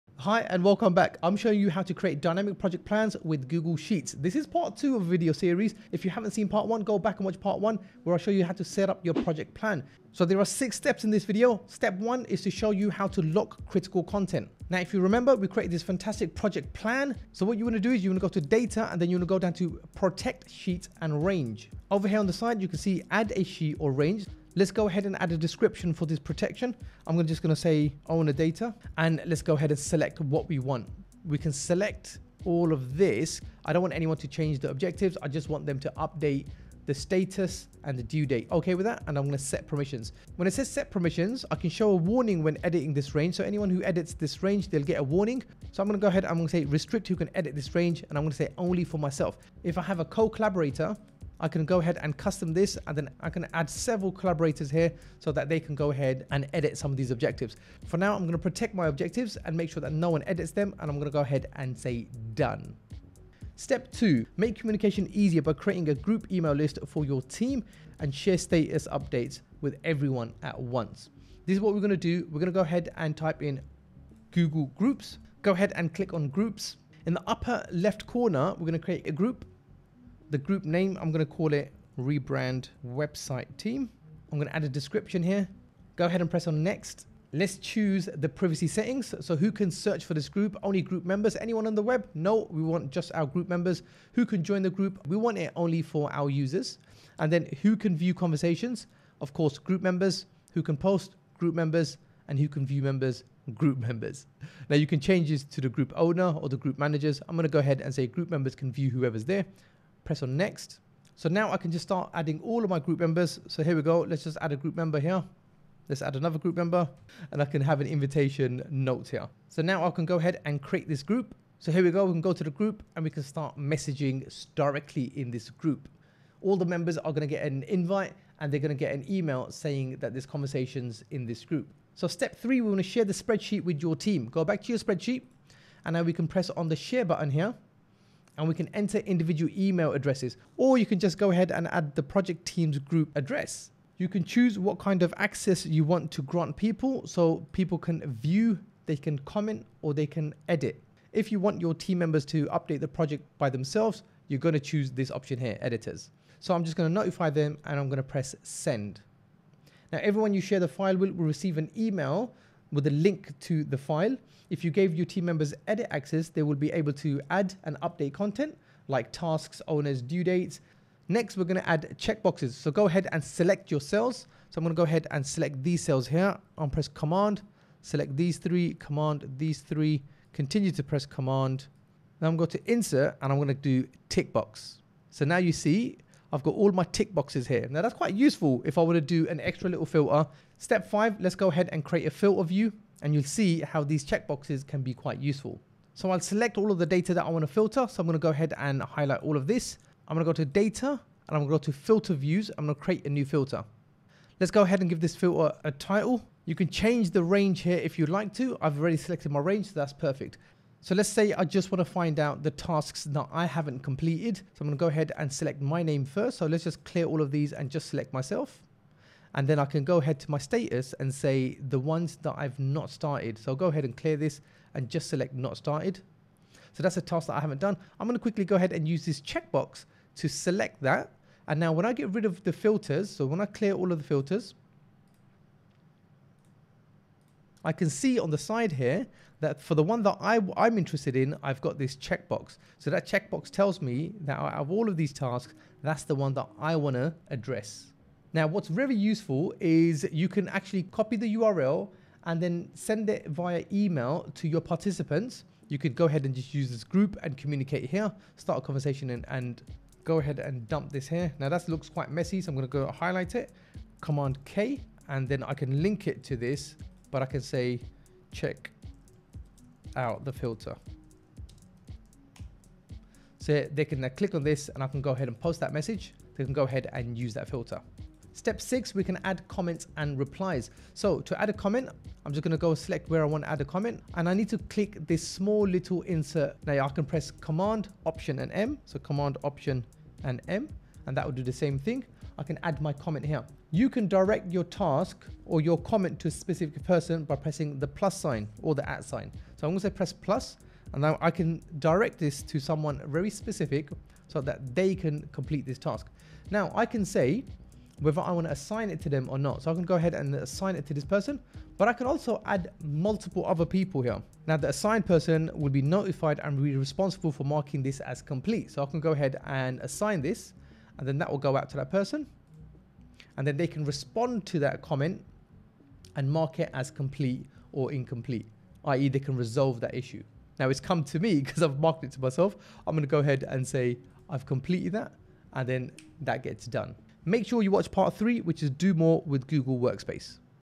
6 Steps to Collaborate with Google Sheets
Create dynamic project plans with Sheets
Part 2. Collaborate with your team in Sheets

0:00 Intro
0:13 1 Lock critical content
1:03 2 Create a Groups email list for your team
2:12 3 Share the spreadsheet with your team
2:50 4 Add checkboxes
3:10 5 Create a filter view
4:37 6 Add comments and replies